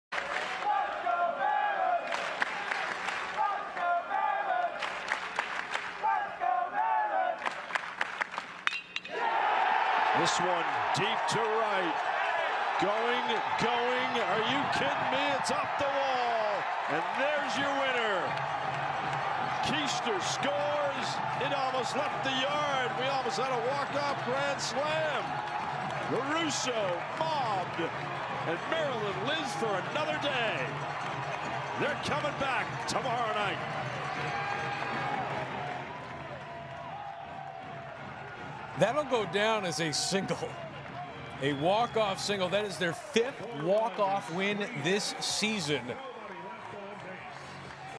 Maryland hits 11th inning walk-off to stay alive in NCAA regionals
Maryland's Nick Lorusso blasted a deep fly ball into right-center field to plate the winning run in the 11th inning and keep the Terps alive with a 7-6 win over UConn.